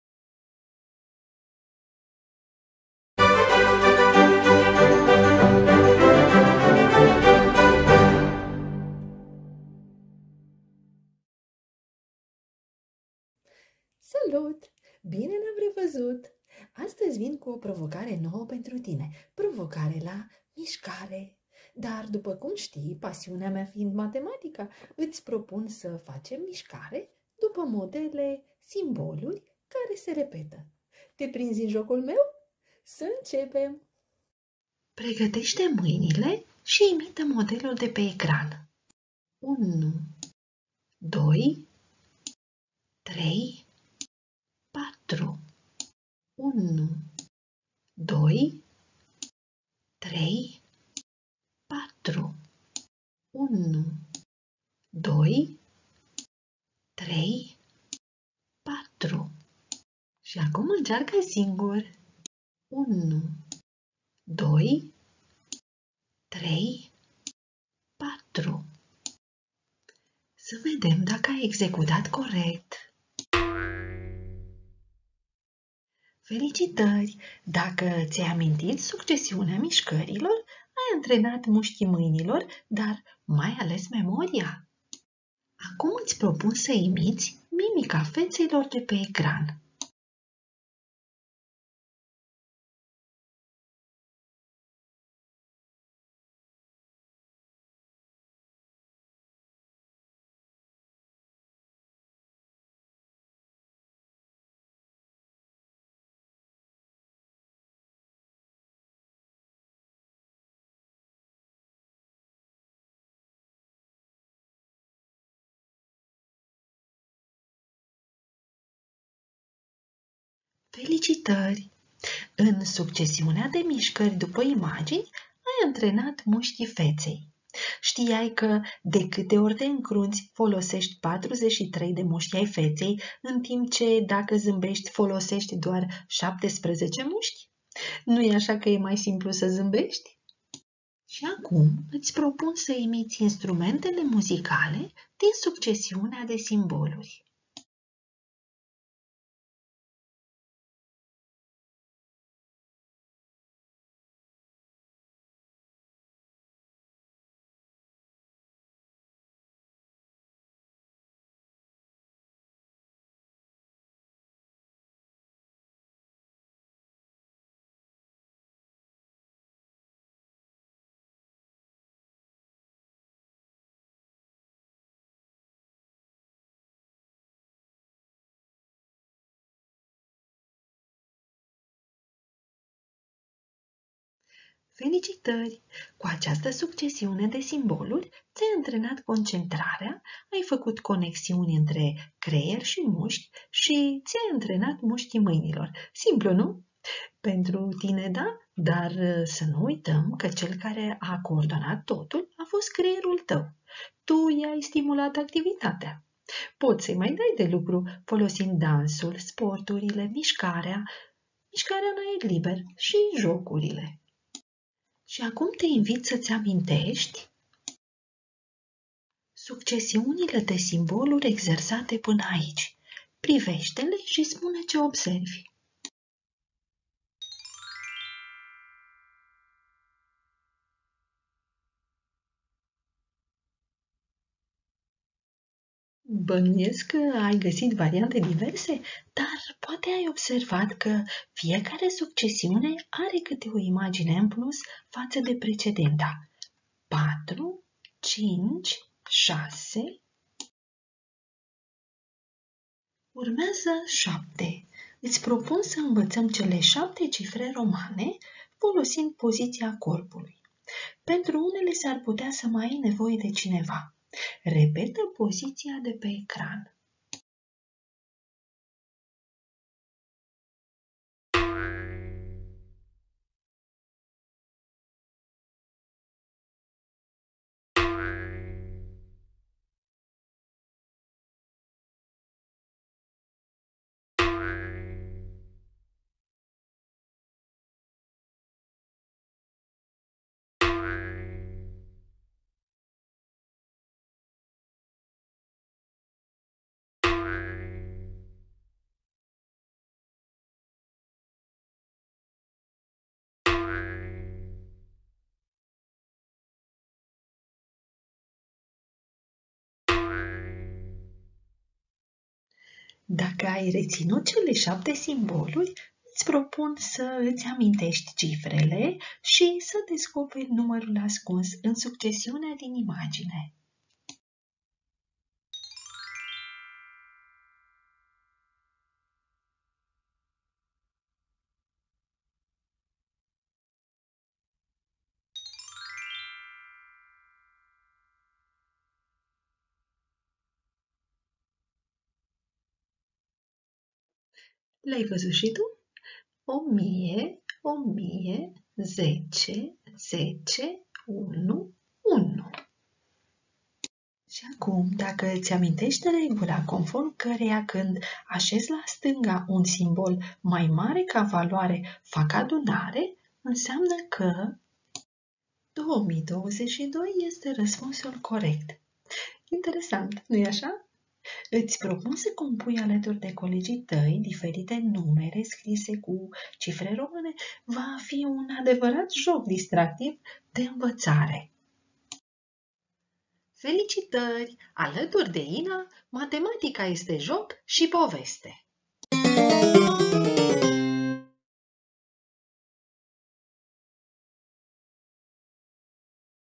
Construirea de regularități simple
Din seria competențelor de identificare a unor relaţii/ regularităţi din mediul apropiat, videoclipul propune construirea de regularităţi simple cu simboluri și cifre romane, respectând una sau mai multe reguli diferite.
Materialul rulează secvențe cu exerciții de mișcare pe care copiii trebuie să le repete, apoi să descopere diferite elemente logice sau primesc informații referitoare la stil de viață activă și sănătoasă. Copiii fac cunoștință cu cifrele romane într-un mod plăcut pe baza exercițiilor de mișcare ce ușurează înțelegerea, realizarea de conexiuni și favorizează învățarea.
În prezentare se folosesc notații specifice scrierii cu citre romane și un exercițiu aplicativ.

Autor: Valentina Ștefan